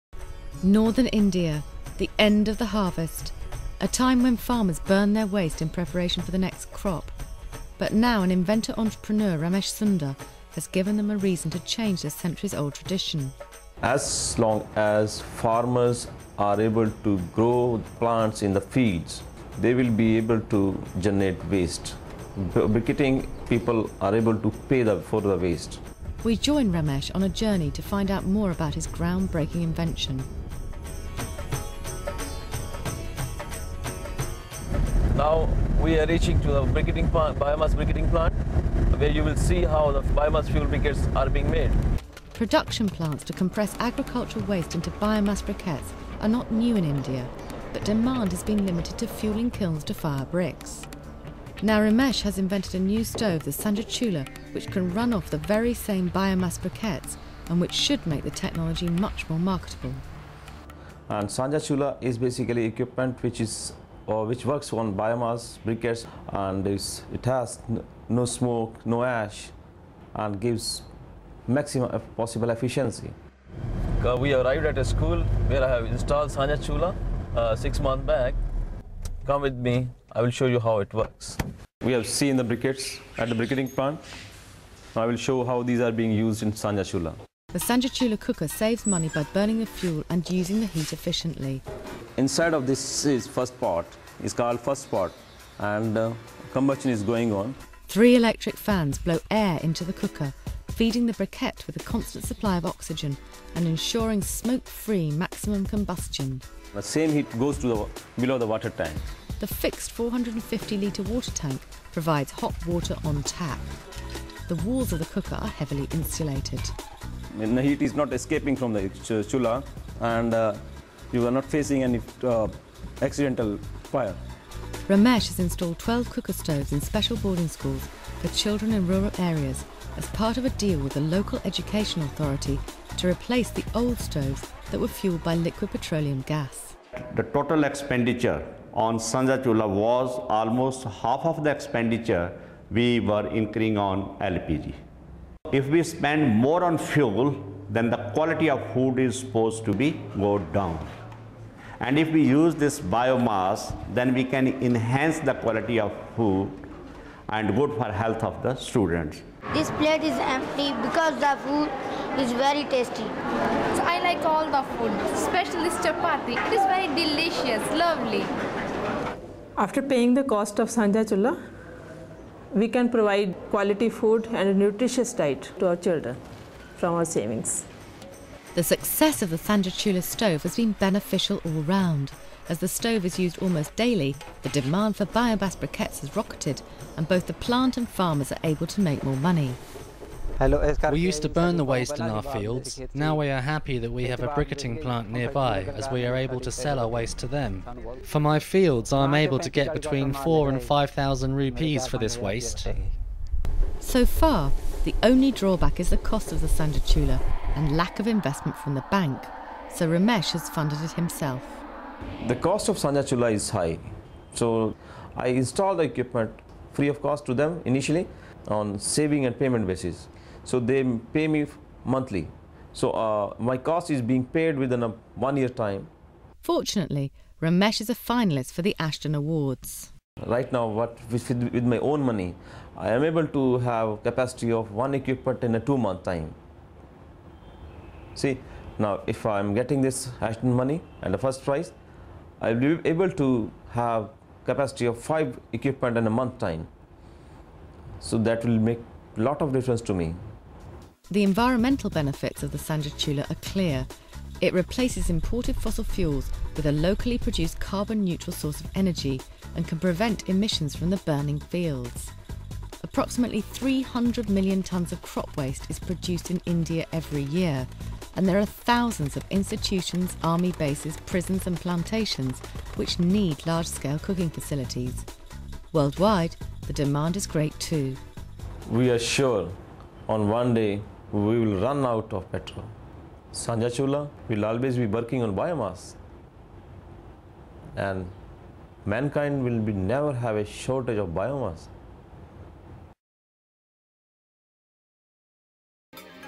Nishant Bioenergy, India, Efficient school biomass cookstoves - Ashden Award winner
Nishant Bioenergy won an Ashden Award for Sustainable Energy in 2005.

Nishant Bioenergy, a small business in Chandigarh, has developed the Sanjha Chulha (combined stove) - a large stove for institutional-scale cooking, which uses briquettes made from crop waste as fuel. Each stove can provide meals for up to 650 people, and thirteen stoves are now in use in residential schools in India, with more on order.

India has many schools and other institutions which provide meals for large numbers of people. Liquified Petroleum Gas (LPG) is widely used for cooking, and is currently subsidised by the Government. However this subsidy is due to be phased out over the next five years and the world price of oil is rising, so the cost of cooking is set to increase.

Crop waste which cannot be used for animal fodder is often burned in the fields, in order to allow a second crop to be planted quickly. India has considerable expertise in compressing this waste into briquettes which can be used as fuel, and there are many commercial briquetting plants in operation. The main market for briquettes is brickmaking: however demand is variable and the price which brickmakers are willing to pay is low because the alternative is cheap coal.

Briquettes are cheaper than LPG for cooking, and the Sanjha Chulha thus solves two problems - it allows schools to use a cheaper, sustainable fuel, and provides the briquetting industry with a more regular and higher-price market. Because schools are unable to afford the initial capital cost, Nishant Bioenergy provides finance which they can repay within 18 to 24 months from their savings in fuel bills.

The Ashden judges were impressed with the commitment and ability of Ramesh Nibhoria, who has built up Nishant Bioenergy from scratch. He has developed a stove which brings multiple benefits, and a financing package which makes it affordable. Cooks like to use it and their institutions save money. Greenhouse gas emissions and local pollution are reduced. The fuel supply chain brings secure income to briquette-makers and the small farmers who supply them with crop waste. There is great potential for replicating this technology throughout India and elsewhere.